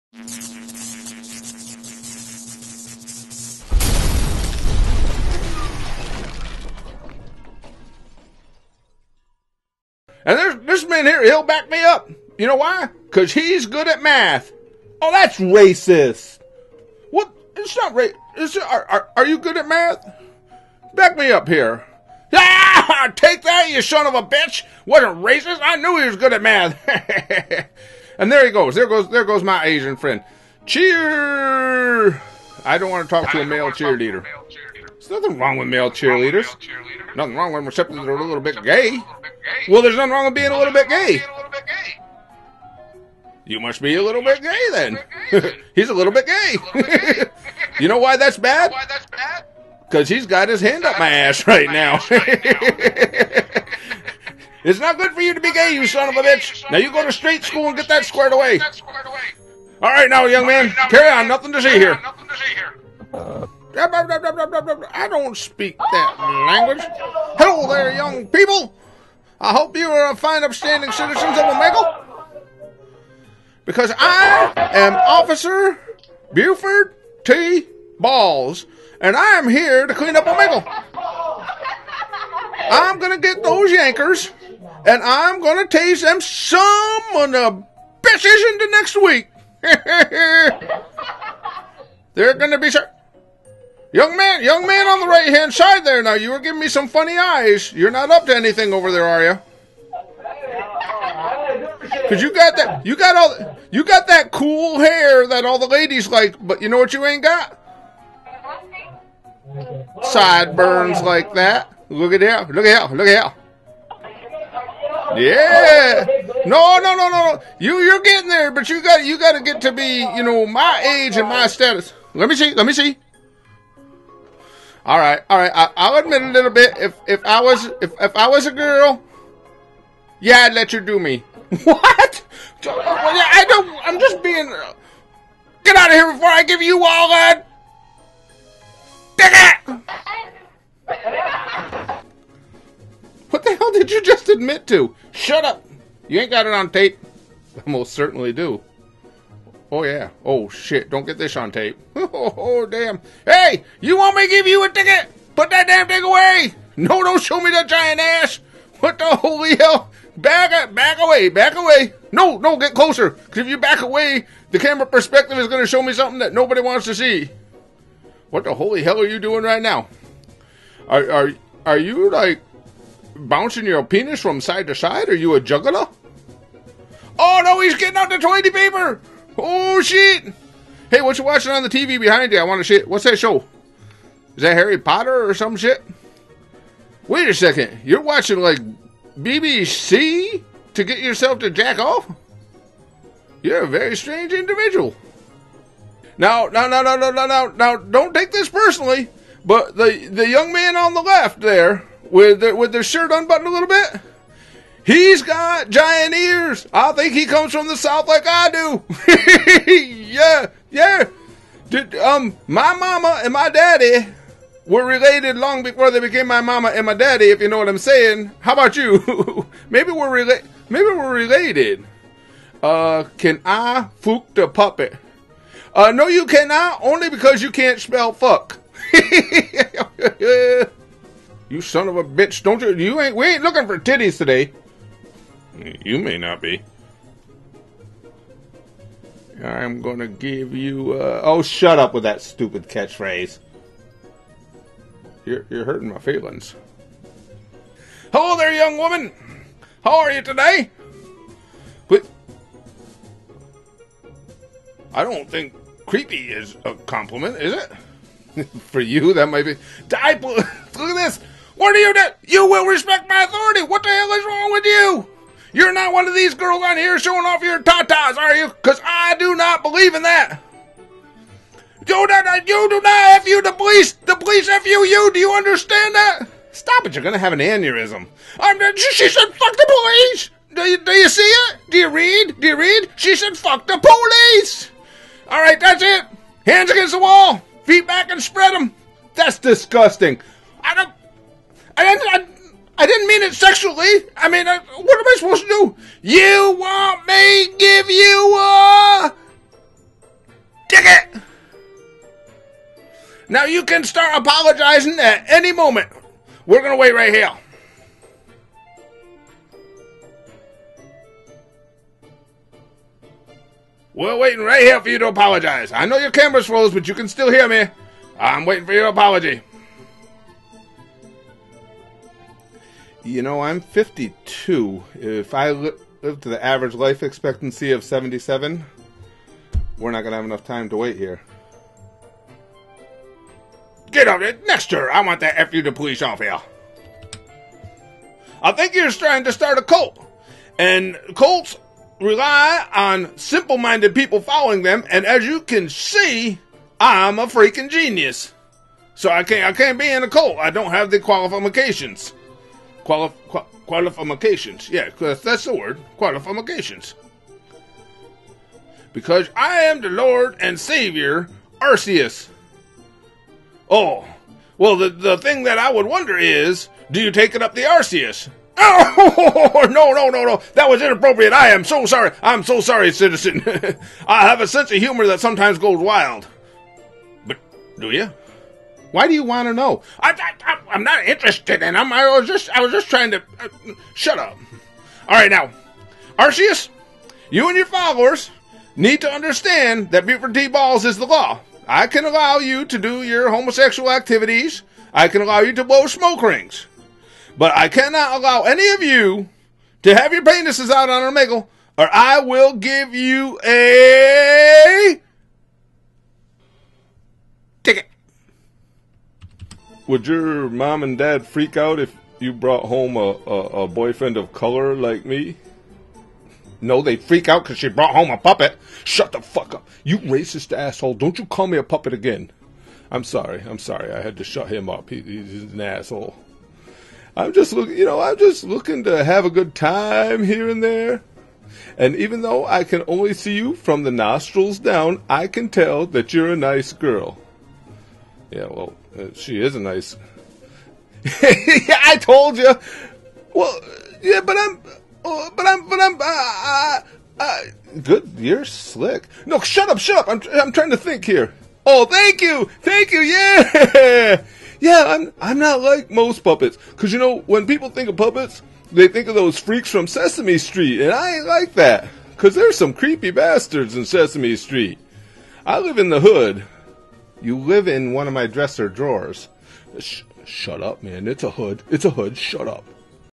Omegle Trolling Funny Chat Roulette | Buford Impresses Everyone?
Omegle trolling some funny chat roulette! Buford T. Balls stumbles upon a wide variety of Omegle folks... Do you suppose they were all adequately impressed?!  :-D

VINE: Search for "Curtis Candy"